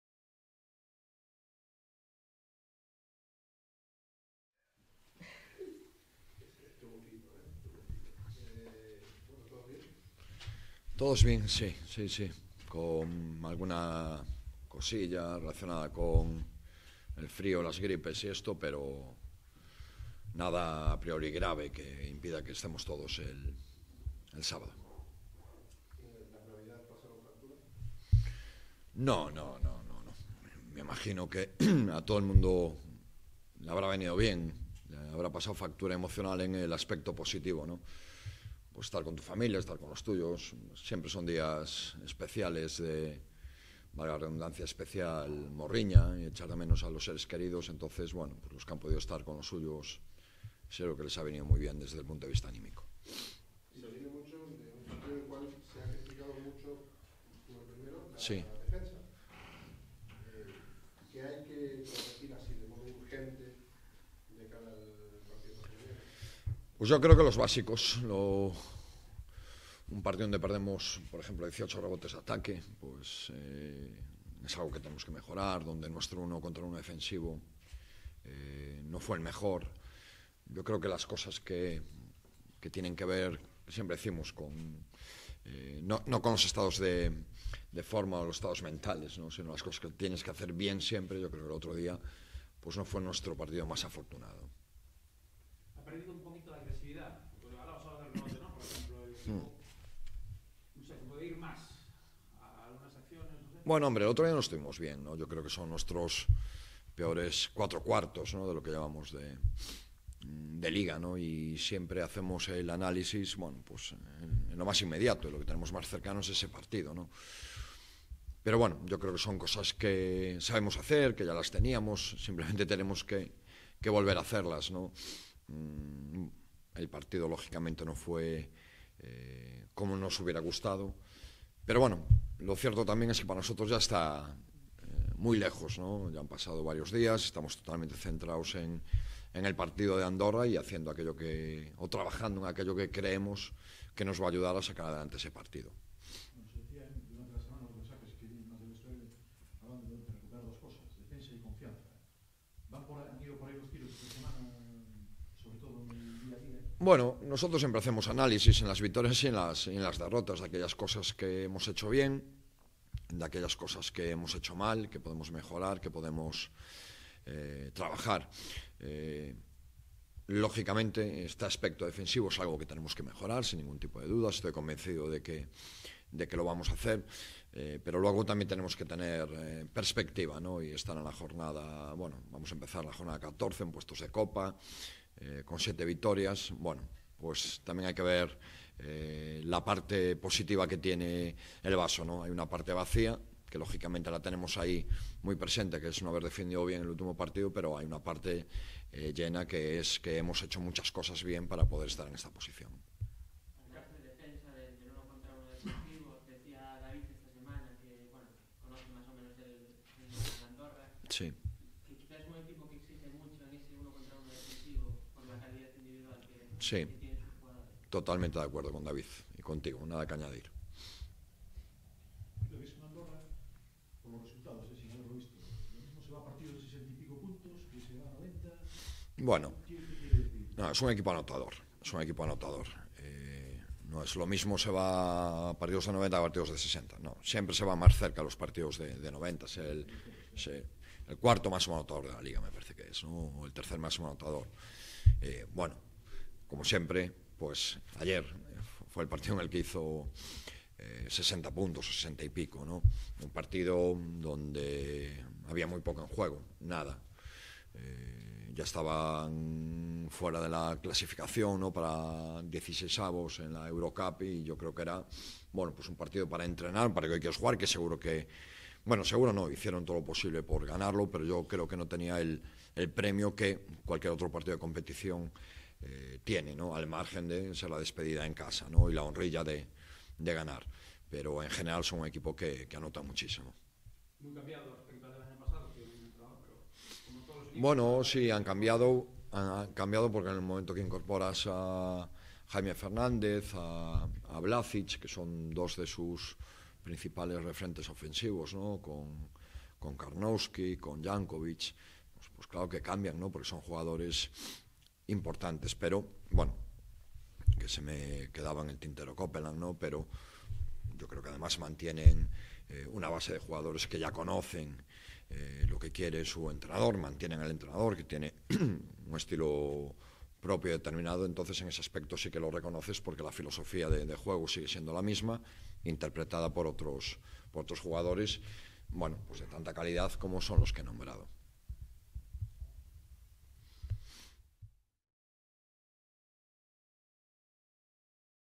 1718 Previa J14: Monbus Obradoiro - MoraBanc Andorra
Rueda de prensa de Moncho Fernández previa al partido contra el MoraBanc Andorra en la jornada 14 de la Liga Endesa (Temporada 2017/18).